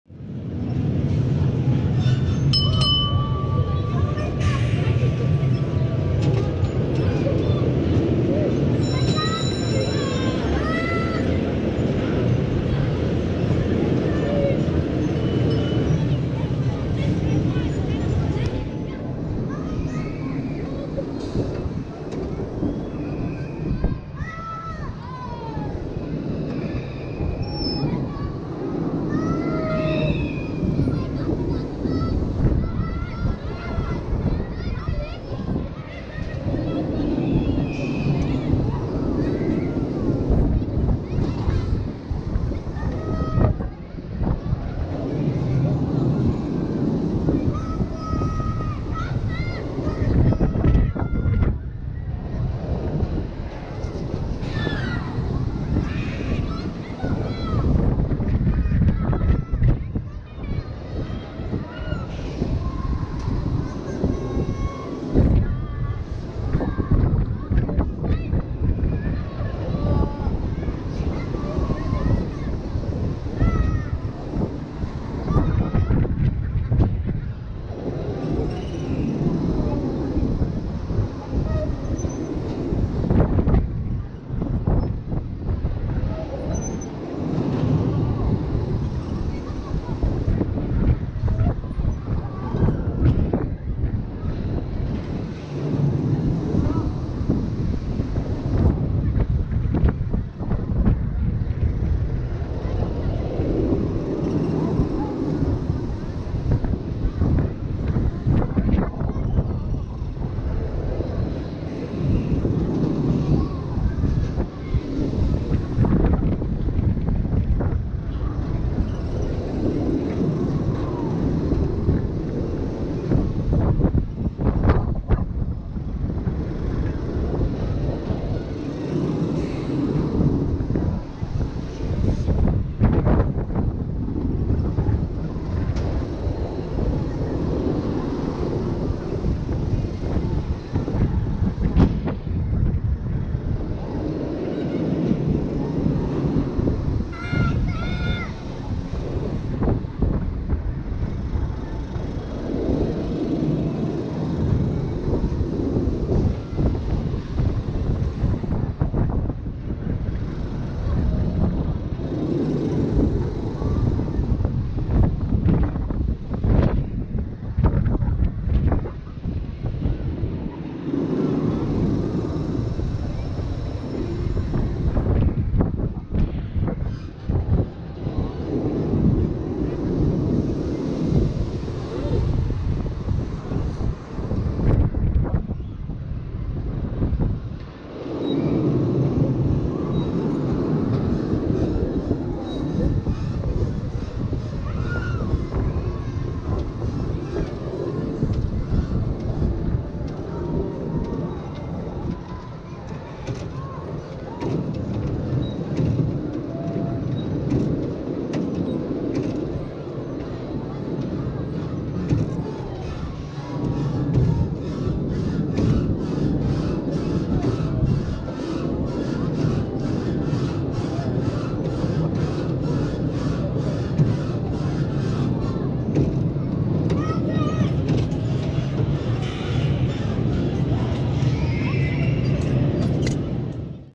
Chance & Morgan Aviator Splat-O-Sphere Movie Park Germany 2018 POV Onride
# SplatOSphere